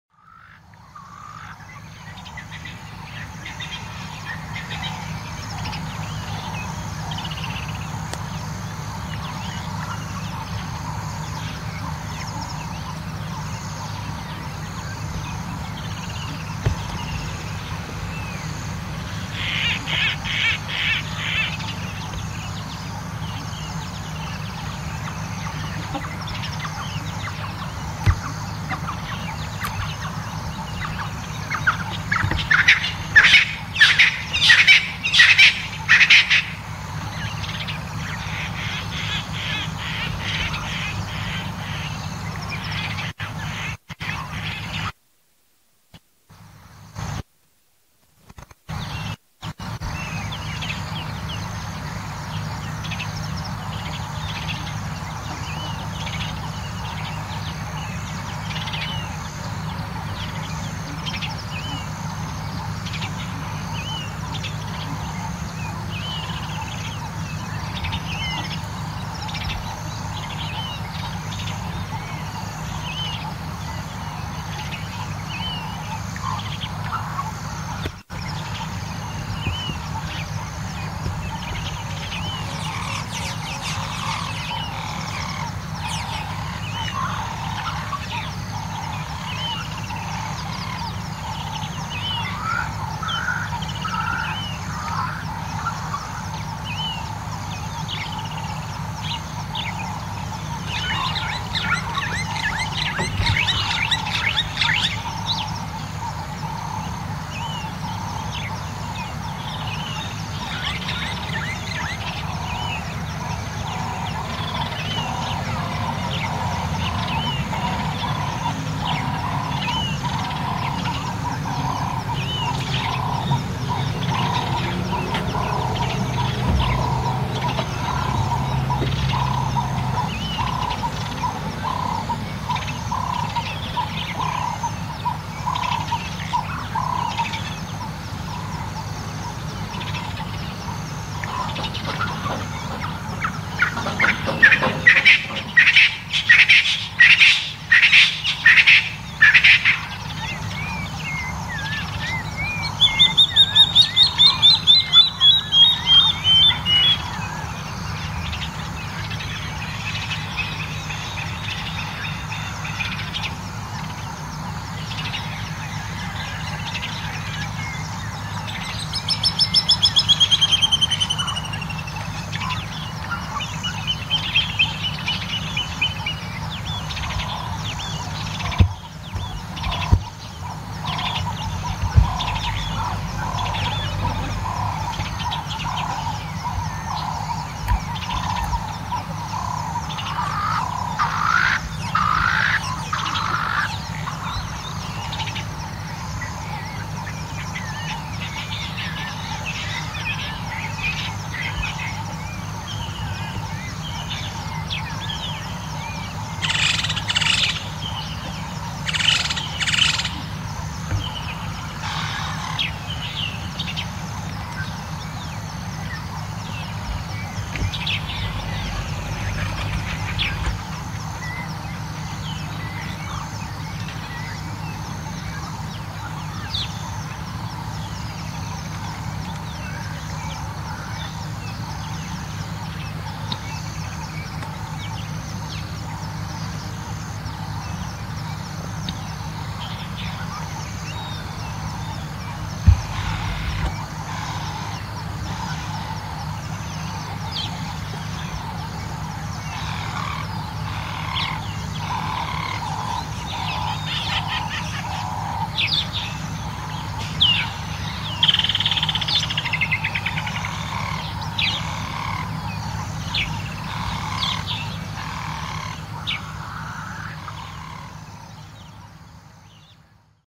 Can you help identify all the birds you hear in this video?
GEAR:
3M Selfie Stick
4TB Lacie SSD
2TB Samsung SSD
256GB Micro SD Cards Can you help identify all the birds you hear in this video?

*The Kruger-concert in the early morning with so many different species of birds heard while watching some wildebeest grazing.*